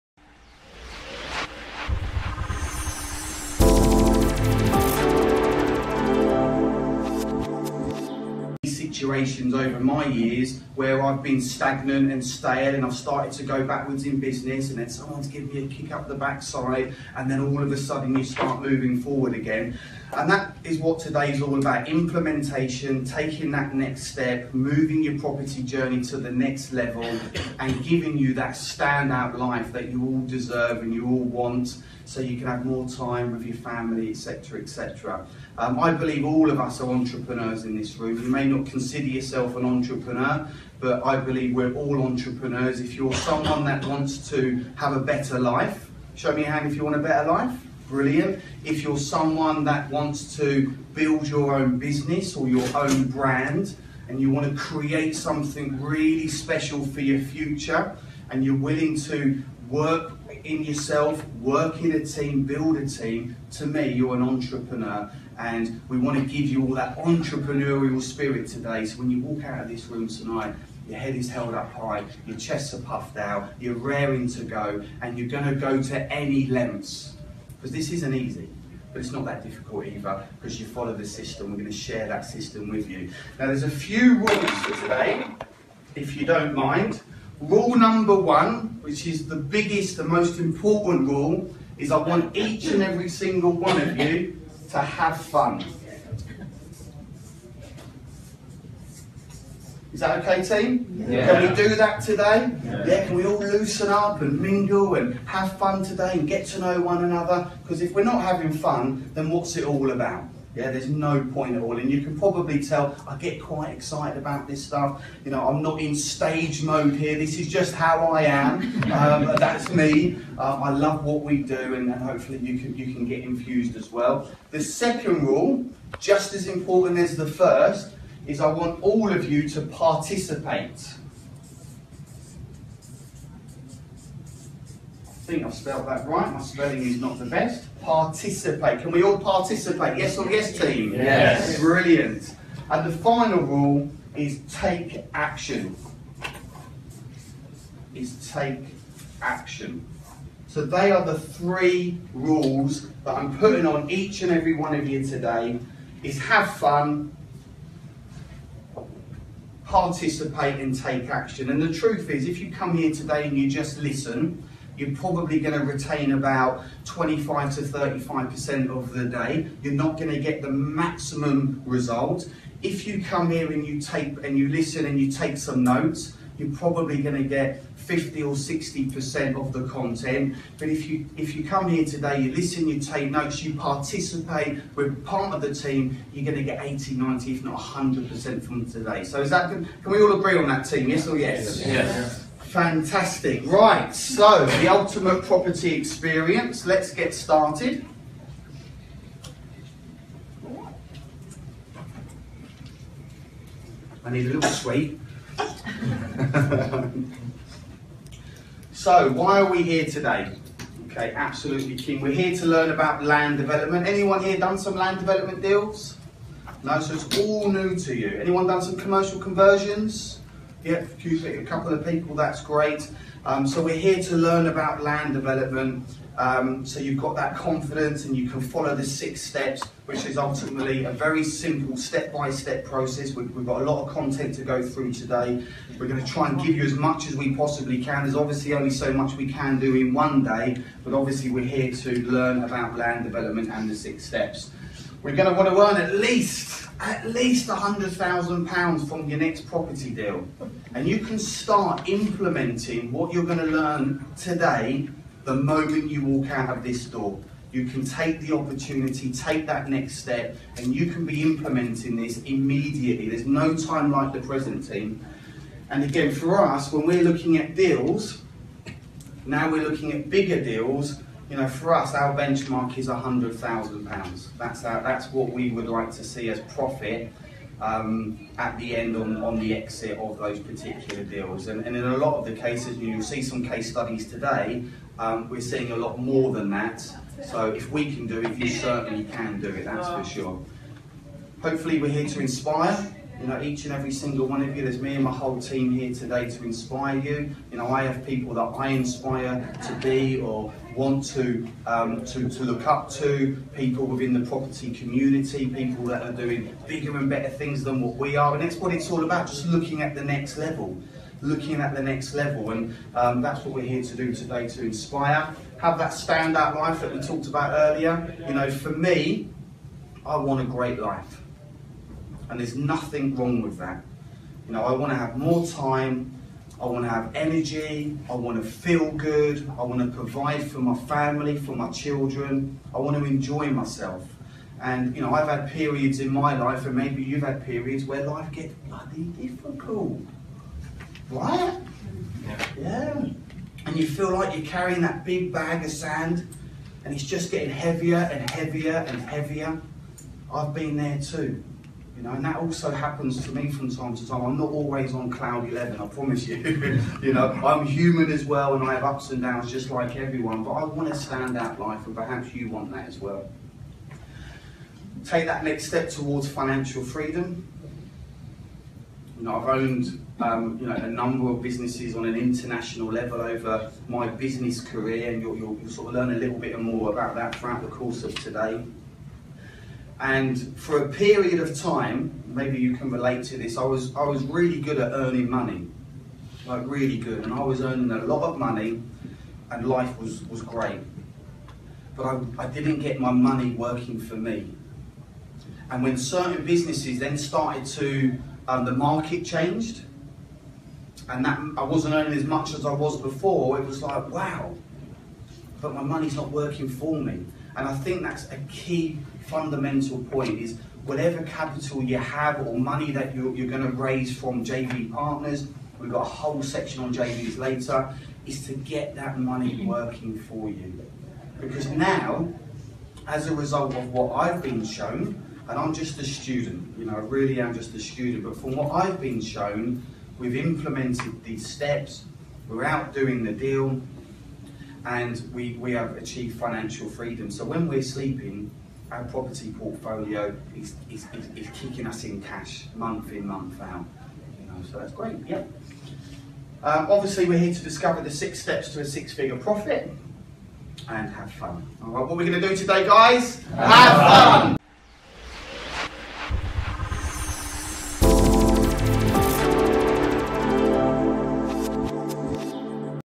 Implementation and Mindset Tips to Invest In Property | Liam Ryan, Assets For Life
In order to become a successful property investor, you will need to become a master of implementation and mindset in your business. One of the best ways to do this, is to set yourself a routine.

When in the day are you going to put in that extra work you need to put in? Do you have a mentor to hold you accountable?

In the video at 2:11 we talk about participating. The question is, are you participating as much as possible in your business?

If you are falling short somewhere, see if you can find a mentor to help or it could be that you need to look at outsourcing certain tasks in your business.

Some things that have really helped us at Assets For Life to keep our mindset on track is personal development. Are you spending enough time constantly learning new things, keeping your mind active and thinking of new ideas?

Resources such as Audible are great for listening to business and mindset podcast for a very low price. It's a highly recommended resource you must consider if you want to stay on track, especially if you're having a bad day.

Struggling with how to implement ideas into action? Need some more guidance on developing a success mindset?